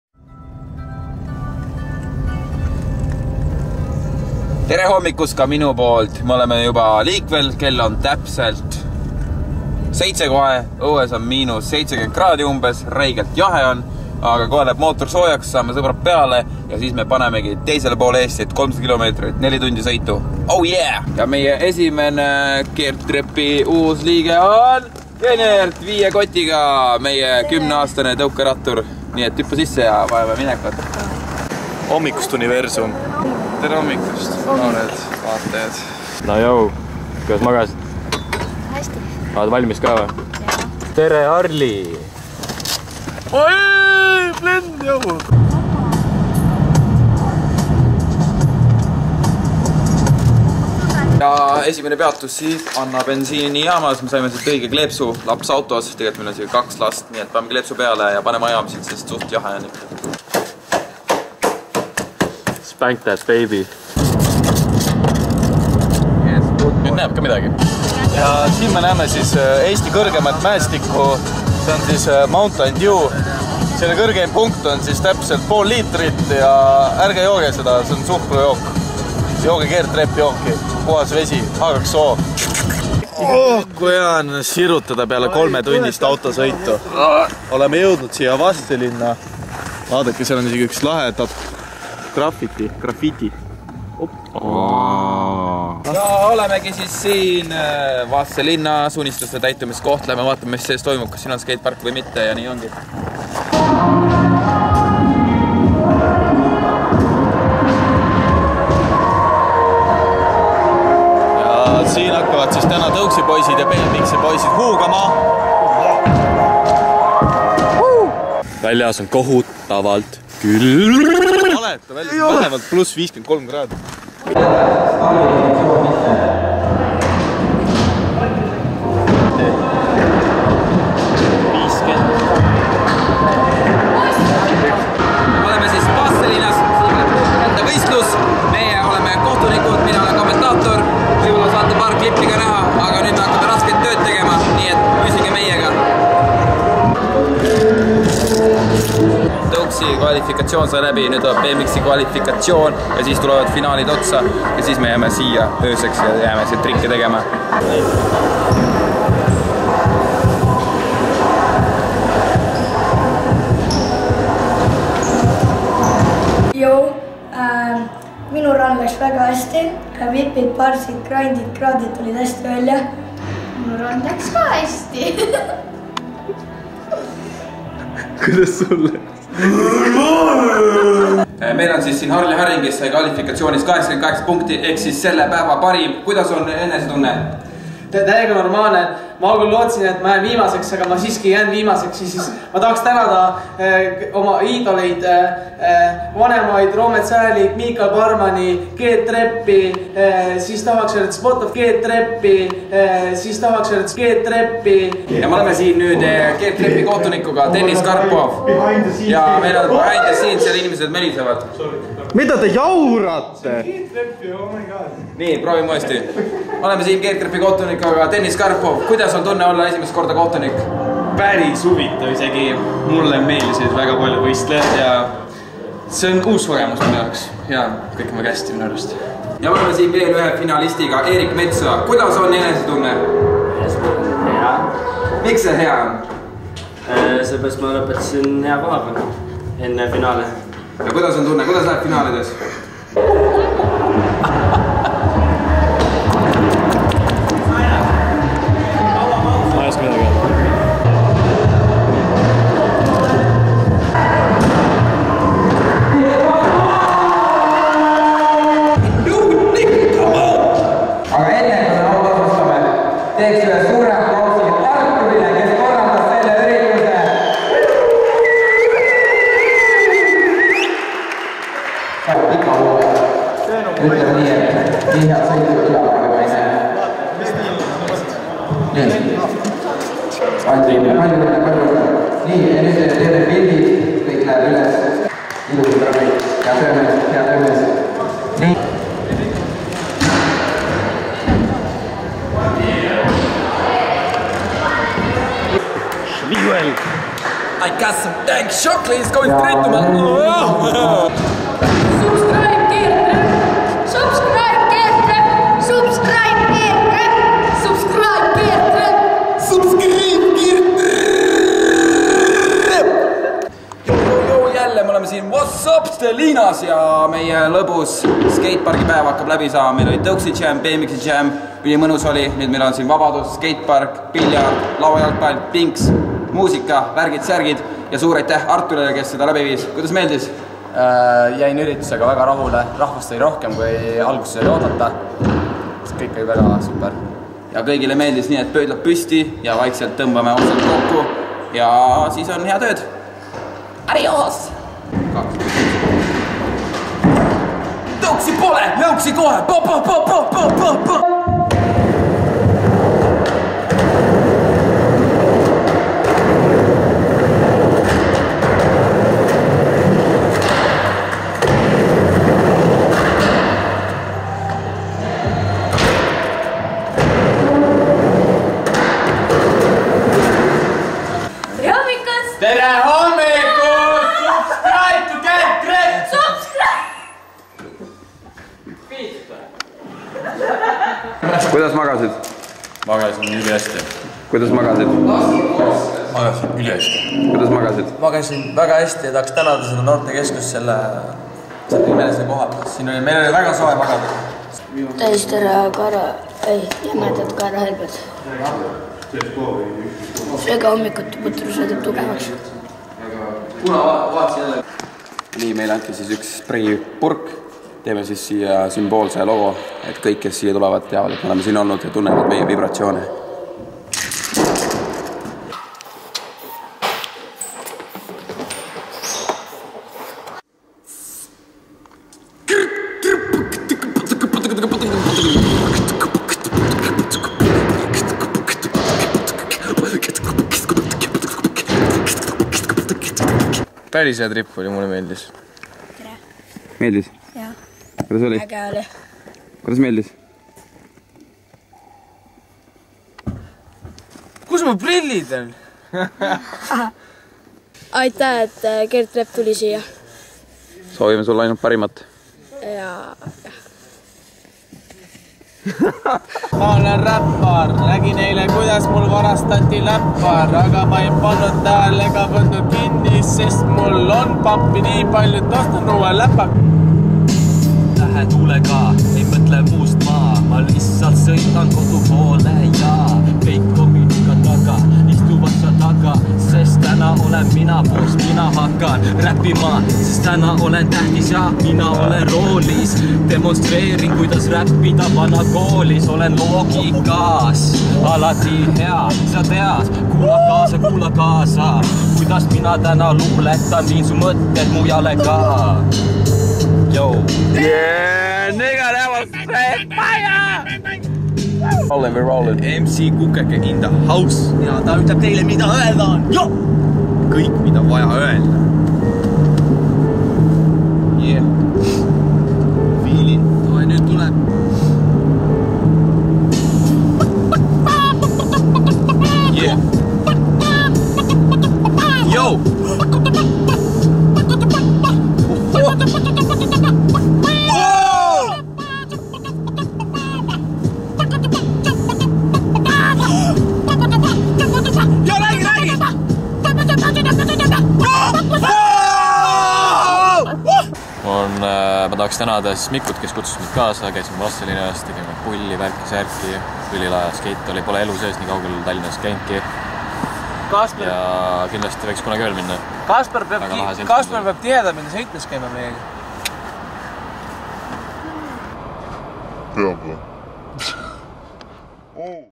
VASTSELIINA VÕISTLUSE VLOG
Siin on siis meie väljasõidu VLOG. Meil oli väga vinge ja kirju seltskond seekord. Sõit oli pikk, aga totaalselt väärt seda. Sõiduklippe me siia videosse ei pannud, varsti tuleb eraldi sõidu video ;)

Aitähh kõigile ja ärge unustage LIKEda, SUBida ja SHAREda!

KRDTRP!